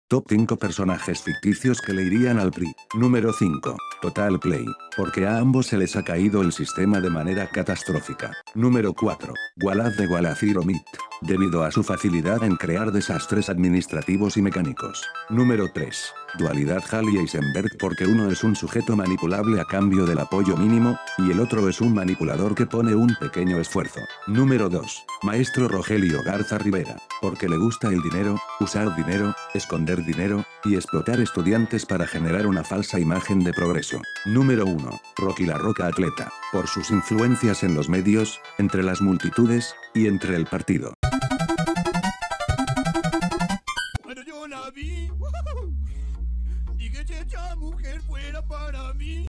TOP 5: Personajes ficticios que le irían al PRI (PRIístas) | Cacaposteo
Este video no tiene como propósito levantar falsos, ni hacer especulaciones ni hacer burla al partido ni a sus militantes ni a los personajes ficticios.
Solo es una parodia de los estereotipos de oposición que últimamente he escuchado de personas ajenas, y no pretende interponerse en las elecciones anuales.

Si tienes otra idea de un top 5, déjala en los comentarios.
Prometo que para el próximo video NO utilizaré loquendo.